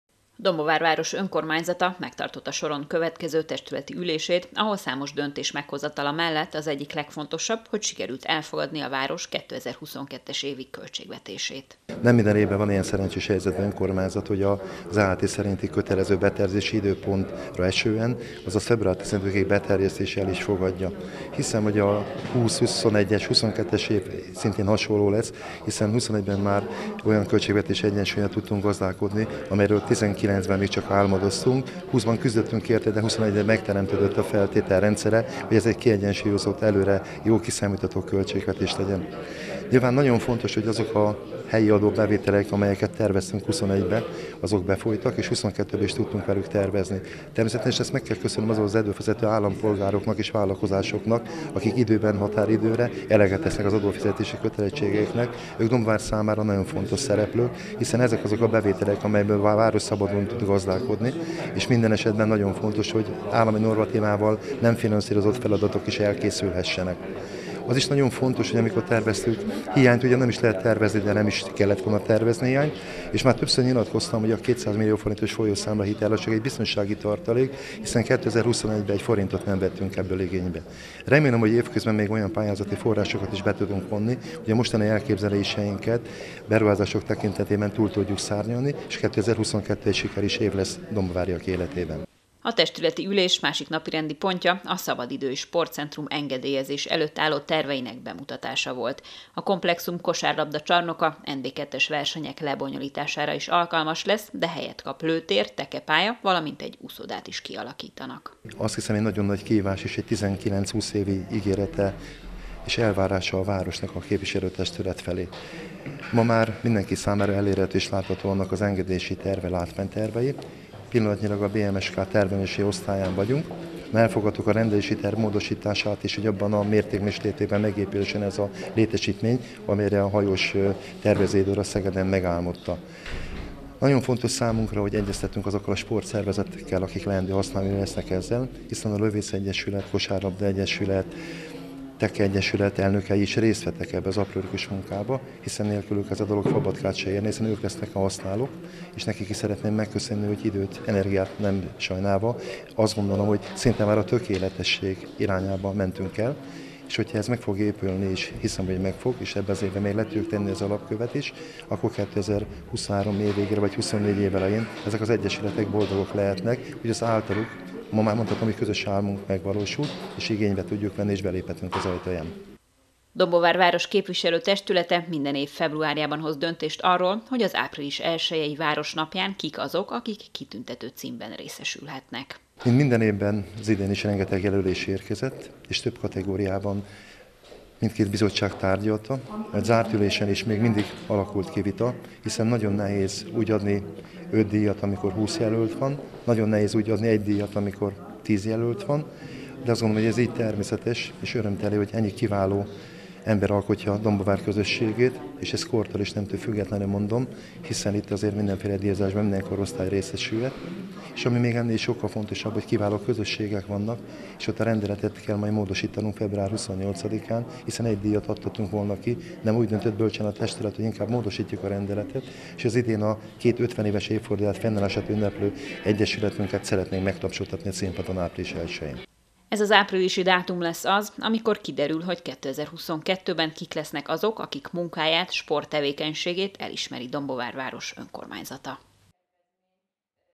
Testületi ülés Dombóváron - elfogadták a költségvetést
Dombóváron a Szabadidő- és Sportcentrum engedélyezés előtt álló terveiről lehullt a lepel, hiszen többek között ez is szerepelt a testületi ülés napirendi pontjai között. Ami pedig a legfontosabb a település életében, hogy a testület elfogadta a város 2022-es évi költségvetését.